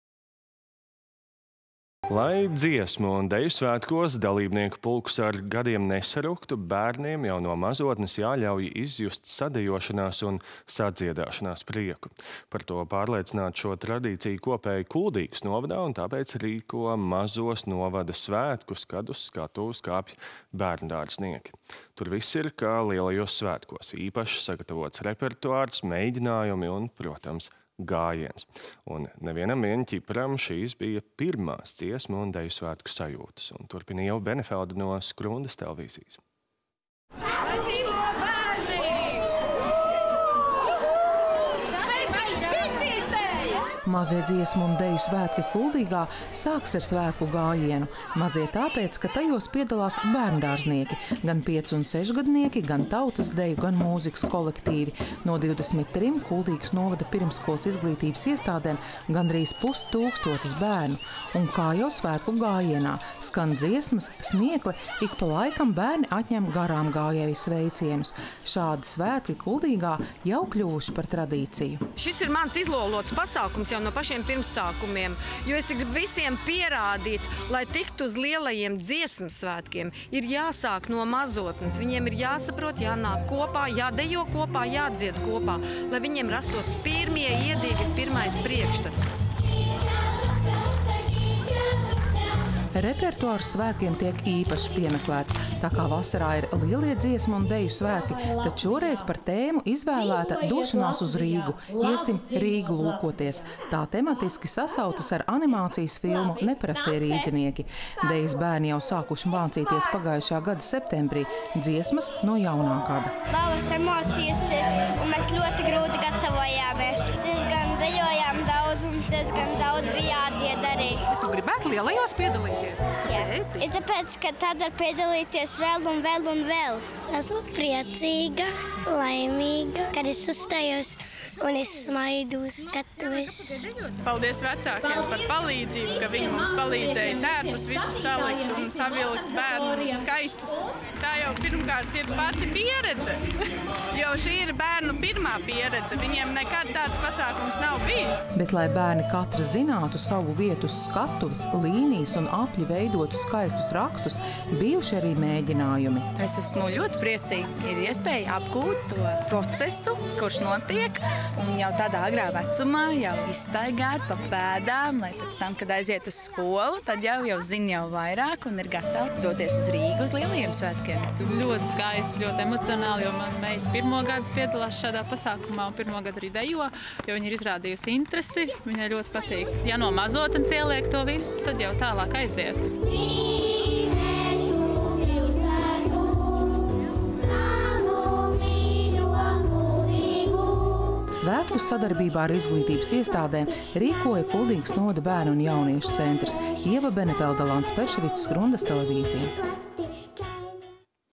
Mazie dziesmu un deju svētki Kuldīgā
Lai Dziesmu un deju svētkos dalībnieku pulks ar gadiem nesaruktu, bērniem jau no mazotnes jāļauj izjust sadejošanās un sadziedāšanās prieku. Par to pārliecināti šo tradīciju kopēji Kuldīgas novadā un tāpēc rīko mazos novada svētkus, kad uz skatuves kāpj bērndārznieki. Tur viss ir kā lielajos svētkos – īpaši sagatavots repertuārs, mēģinājumi un, protams, gājiens. Un ne vienam vien ķiparam šīs bija pirmās dziesmu un deju svētku sajūtas. Turpina Ieva Benefelde no Skrundas televīzijas.